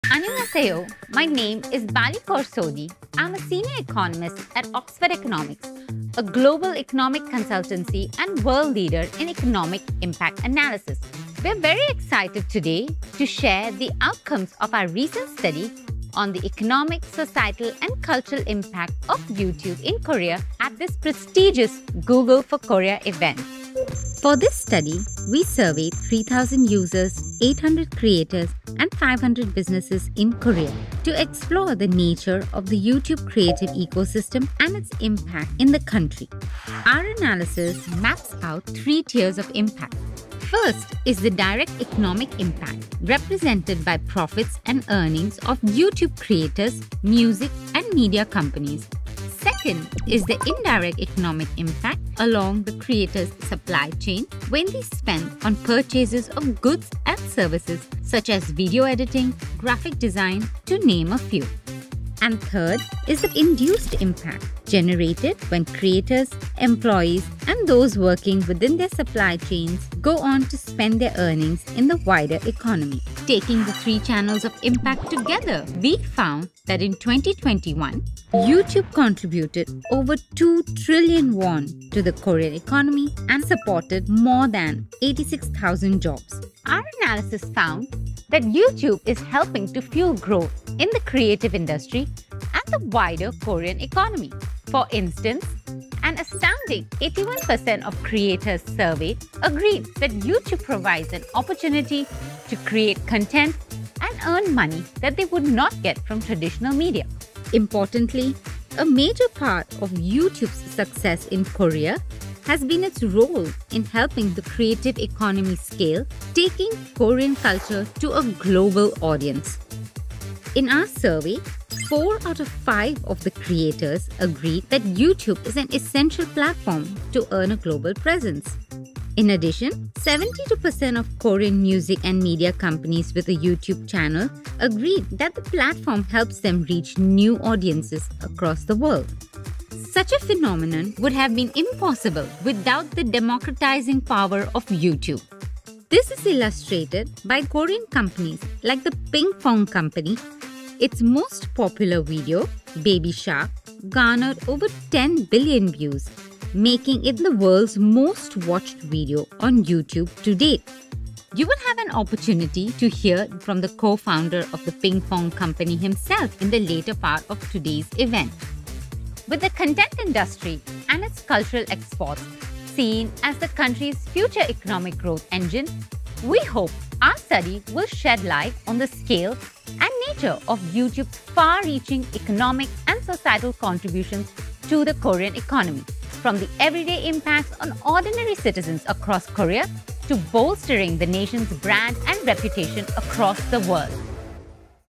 Google for Korea 2022 ｜YouTube Economic Impact Report
Google for Korea 2022
Economic Impact Report｜YouTube와 한국 크리에이티브 생태계의 영향력
- Bali Kaur Sodhi, 옥스퍼드 이코노믹스 수석 경제학자

🔽 보고서 전문 보러가기 : Google의 한국 경제 기여 효과에 대해 더 자세히 알아보세요 🔽
(링크 TBU)

Economic Impact Report | YouTube and Its Impact to Korean Creative Ecosystem
Bali Kaur Sodhi│Senior Economist, Oxford Economics